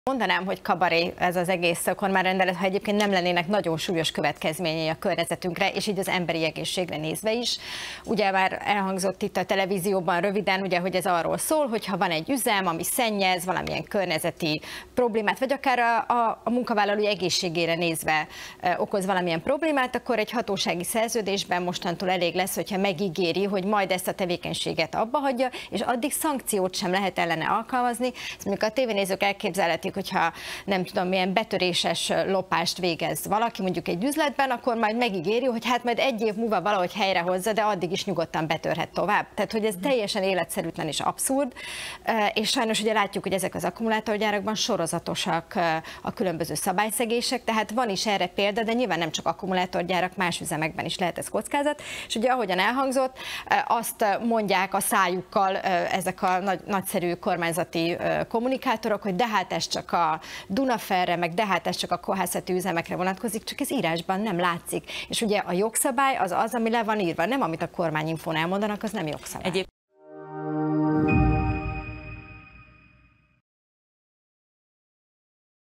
Mondanám, hogy kabaré ez a kormányrendelet, ha nem lennének súlyos következményei
Szabó Rebeka az ATV műsorában

és Instagramon is: @parbeszed_zoldek